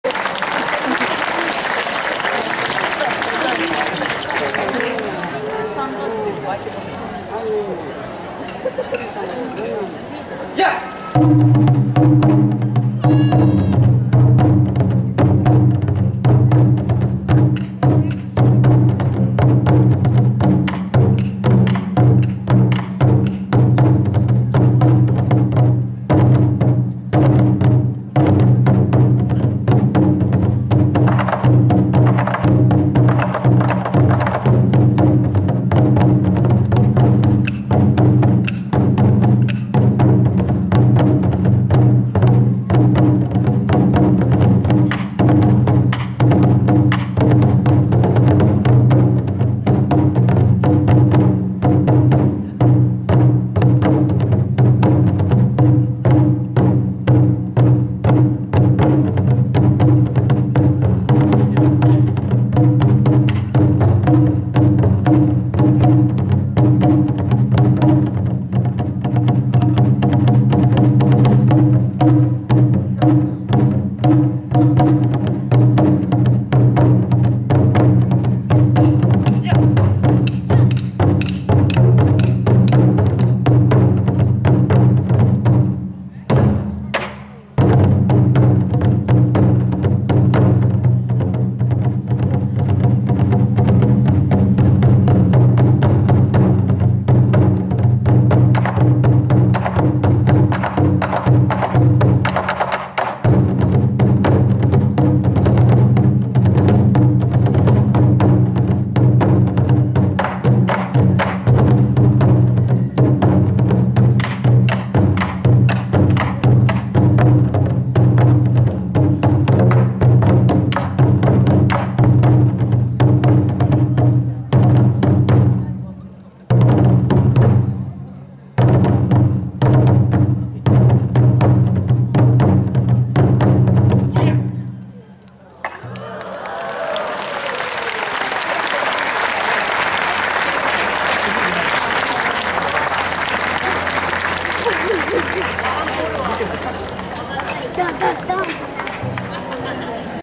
大師よさこいフェスタ2010 西蓮太鼓 奉納式前
10-07-04  西新井大師　大師境内ステージに踊りこのみなさんが集結、奉納式の前に太鼓で景気をつけます。